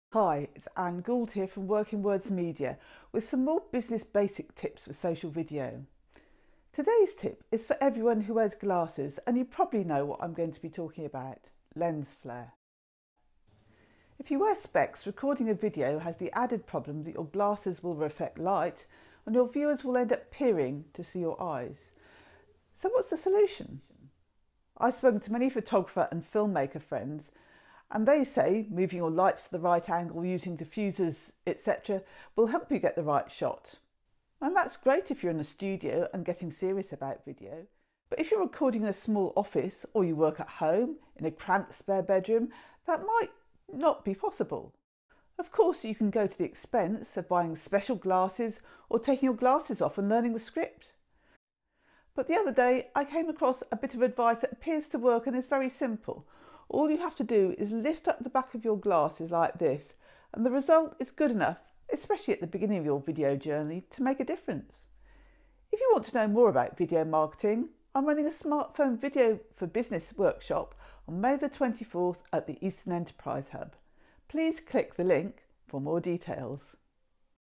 Lens Flare - a solution. Video Basics for Business |WorkingwordzMedia
This video is by Anne Gould, It looks at an easy solution to lens flare for glasses wearers when recording video.
Hi, it's Anne Gould here from Workingwordz Media with some more Business Basics tips for social video.

Today's video is for everyone who wears glasses - and you know what I'm going to be talking about lens flare.

If you wear specs recording a video has the added problem that your glasses will reflect light and viewers won't be able to see your face.

So what's the solution? I've spoken to many photographer and film maker friends and they talk about moving your lights about , using diffusers etc to get the right shot.

And, that's great if you are in a studio.

But, if your studio is a small office or you work at home in a cramped spare bedroom  There may not be room to jiggle about with lights.

Of course you can go to the expense of buying special glasses.

But then I found another bit of advice that appears to work and is very simple.

All you have to do is lift up the back of your glasses like this - and the result is good enough - especially when you are on the beginning of your video journey to make a difference.